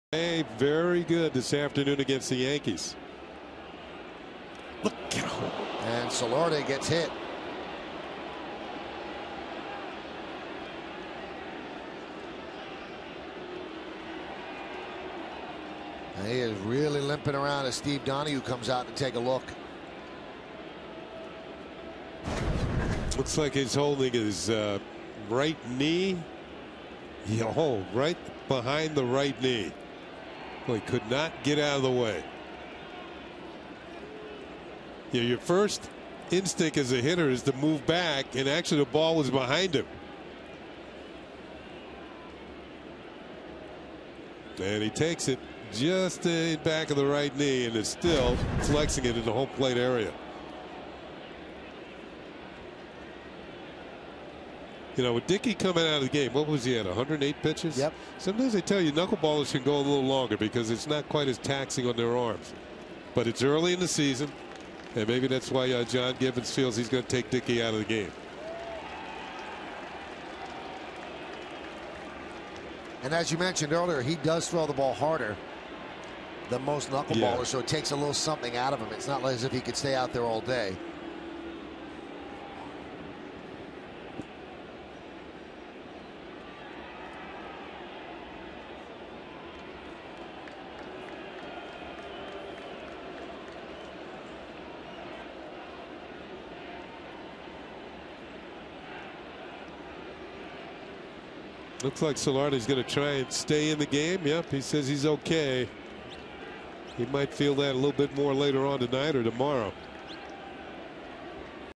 NYY@TOR: Solarte gets hit by a pitch but stays in
4/5/14: Yangervis Solarte gets hit in the leg by an Aaron Loup pitch and is shaken up, but stays in the game after a trainer visit